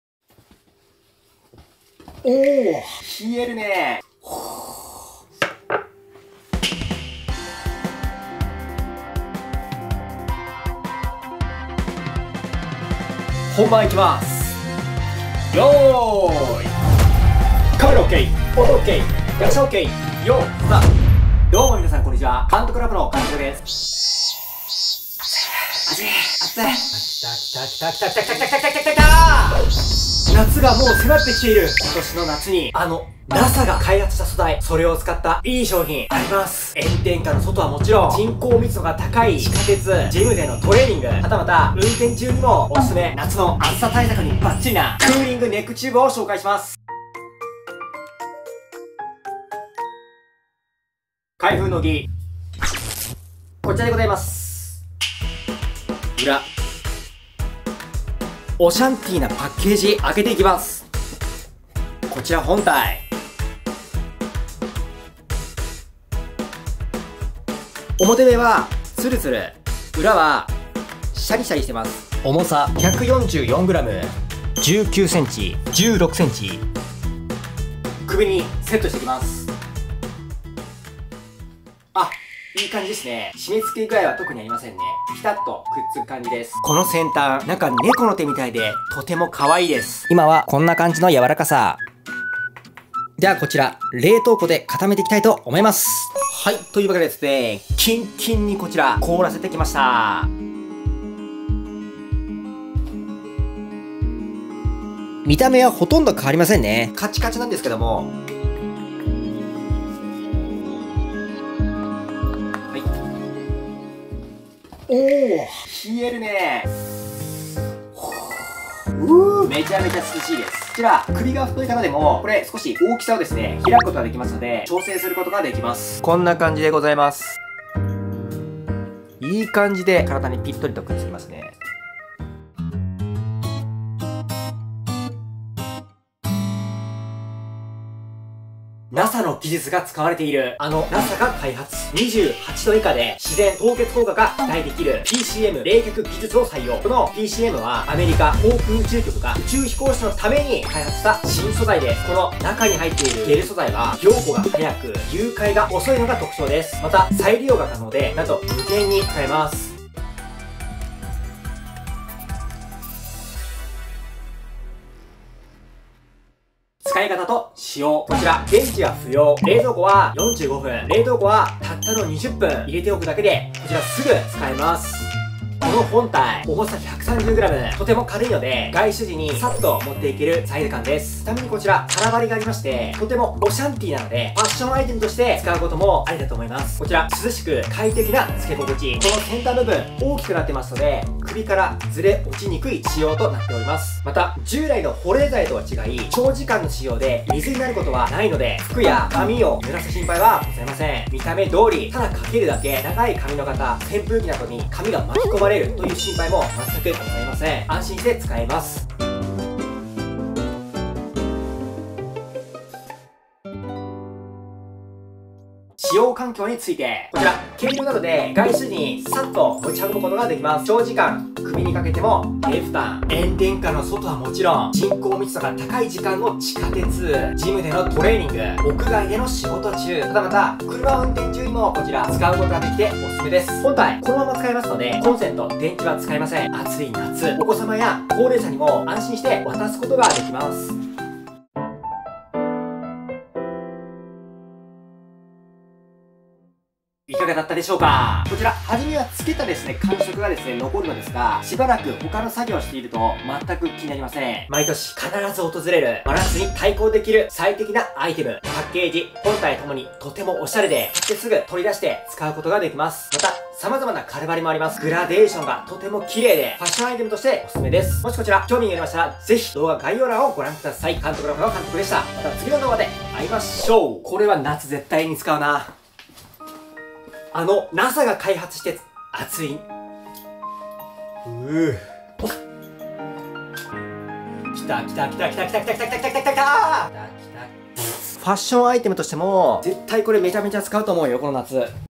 【真夏に耐える】子供、高齢者も安全に使える！冷凍庫たったの20分でひんやり冷却、クーリングネックチューブとは？【XINHOME】
Amazon商品ページ（XINHOME /クーリングネックチューブ）

NASA開発の新素材「PCM」を使用。
炎天下の外は勿論、人口密度が高い時間の地下鉄、ジムでのトレーニング、はたまた車運転中におすすめ。

電源不要で、冷蔵庫は45分、冷凍庫はたったの20分で入れておくだけですぐ使える夏の暑さ対策にぴったりな「クーリングネックチューブ」をレビューします！